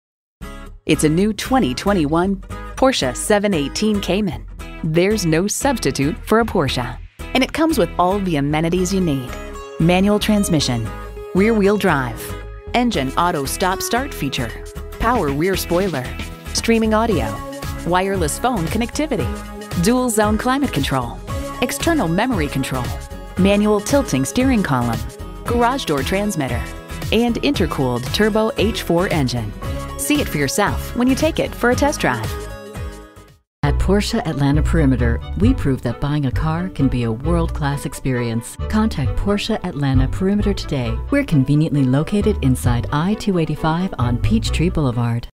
New 2021 Porsche 718 Cayman Atlanta, GA P16979 - SOLD
Year: 2021
Make: Porsche
Model: 718 Cayman
Trim: S
Engine: 2.5L H4 Turbocharged DOHC 16V LEV3-LEV160 350hp
Transmission: 7-Speed Porsche Doppelkupplung (PDK)
Color: Black
Interior: Black
Stock #: P16979
VIN: WP0AB2A89MS275066

Address:
4006 Carver Drive